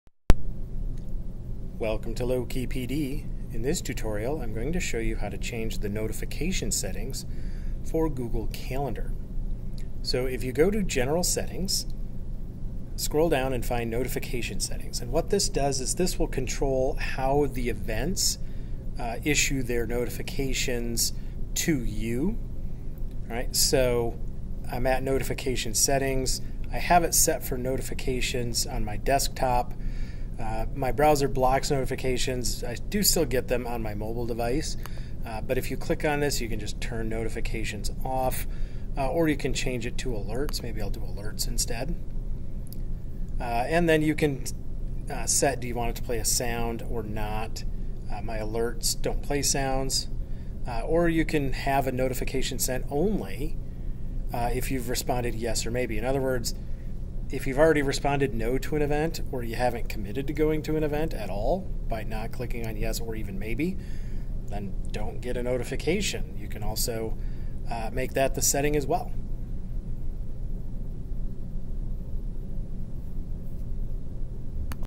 Setting Default Notifications in Google Calendar
If you notice that you're always setting the notifications for your events to the same things, consider making those the default settings for your notifications.

I do not upload on a regular schedule, but I do upload frequently.